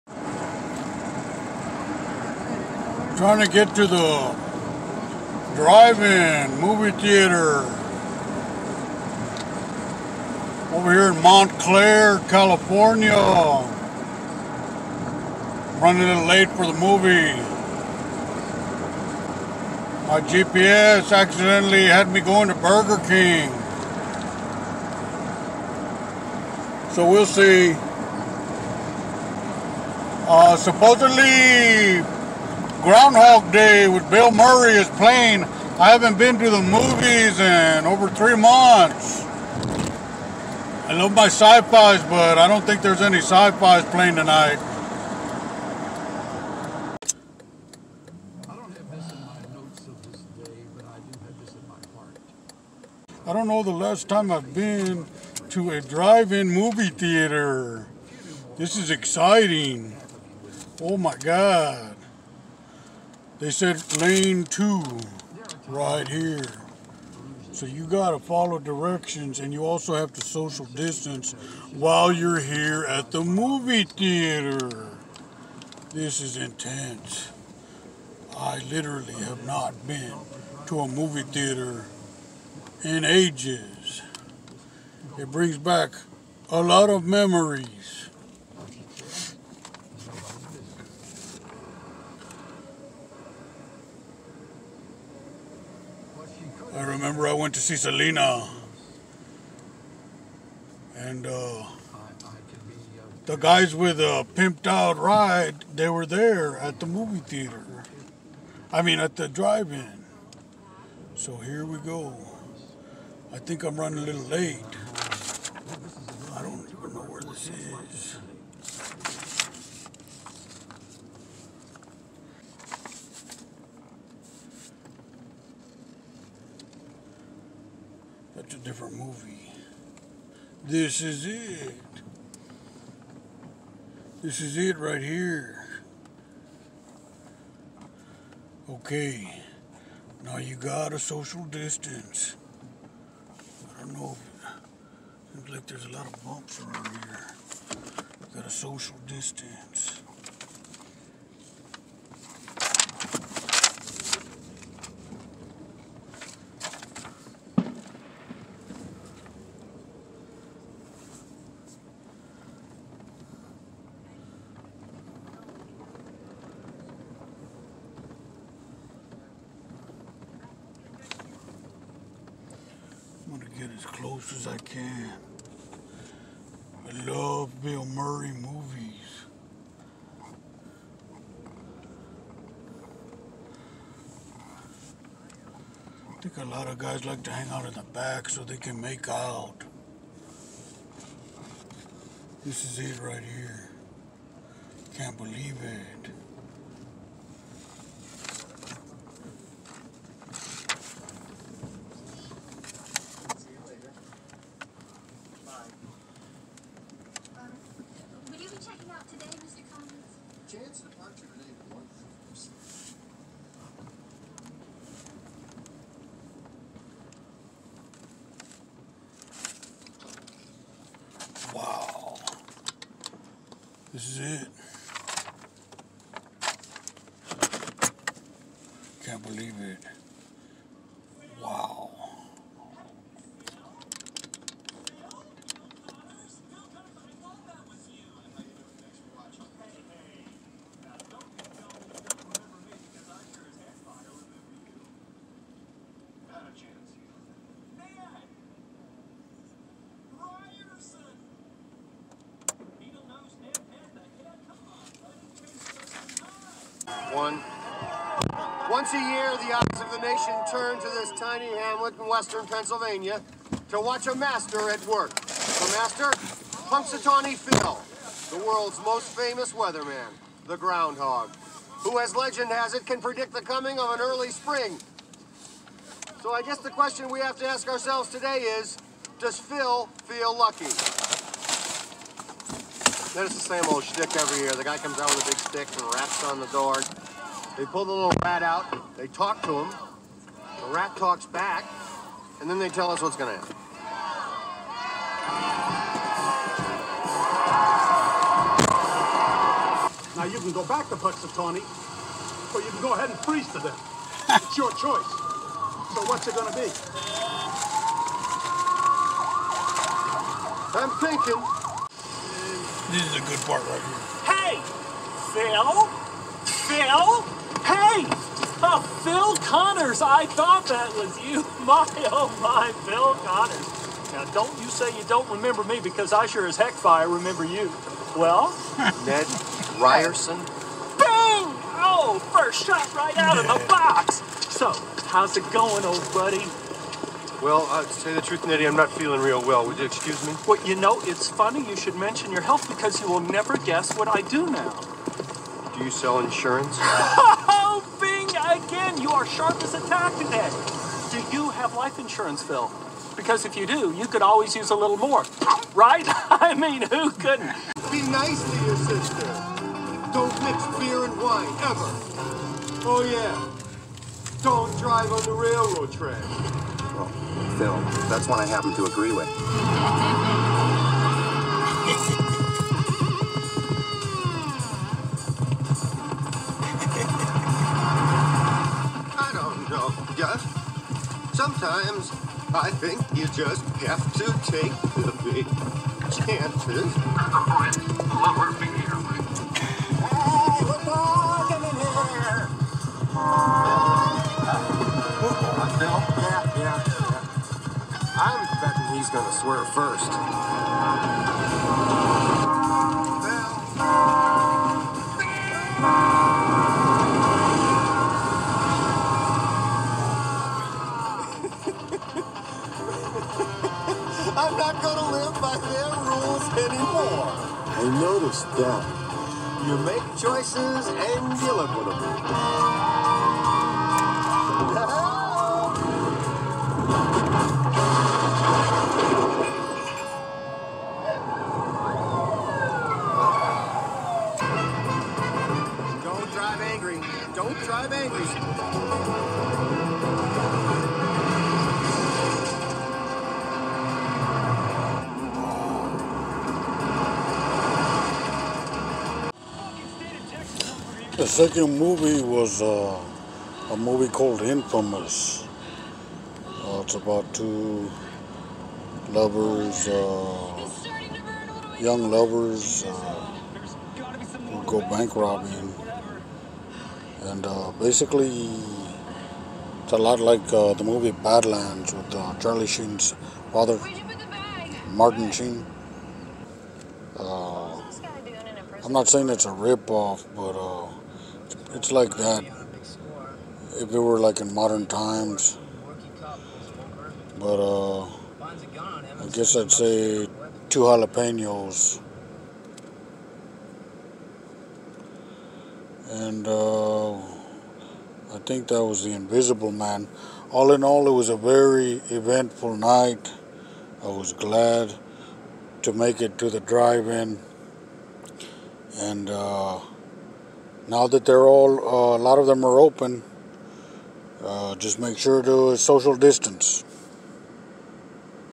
Al Olmos Goes to Drive-In Theatre
Al Olmos here. Got the idea to go to the drive in movie theatre. Heard that they just opened. Apparently this movie theatre in Montclair, CA was open all along, only they practice very strict social distancing. Not since Roger Corman B-movie double bills have Drive in theatres been so popular! It was double bill including an old Bill Murray film and Bella Thorne's latest film about two lovers who start robbing marijuana dispensaries and posting videos of robberies onto the internet making them famous.